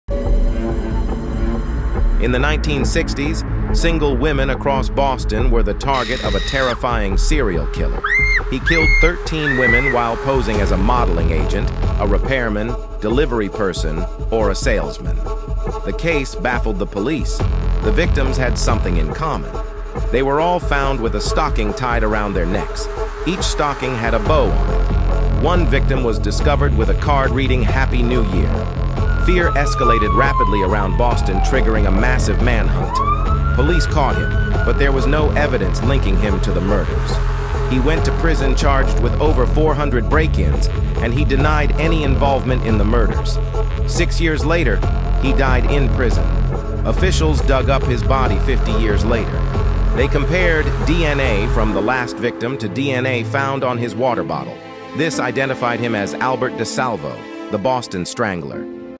😱 Crazy Woman Killer: The Boston Strangler
Discover the shocking true story of the Boston Strangler, a serial killer who targeted single women in the 1960s. Learn how he posed as a modelling agent, repairman, delivery person, or salesman to gain his victims' trust. With each victim found with a stocking tied around her neck, fear escalated rapidly in Boston. Join us as we uncover the chilling details of this case and the massive manhunt that followed.